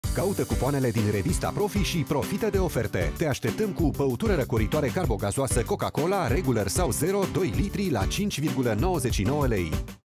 Caută cupoanele din revista Profi și profită de oferte! Te așteptăm cu Coca Cola la un pret special.
Caută cupoanele din revista Profi și profită de oferte! Te așteptăm cu Băutură răcoritoare carbogazoasă Coca Cola regular/zero 2L la un pret special. Oferta supusa unor termene si conditii. Profi, zilnic preturi mici.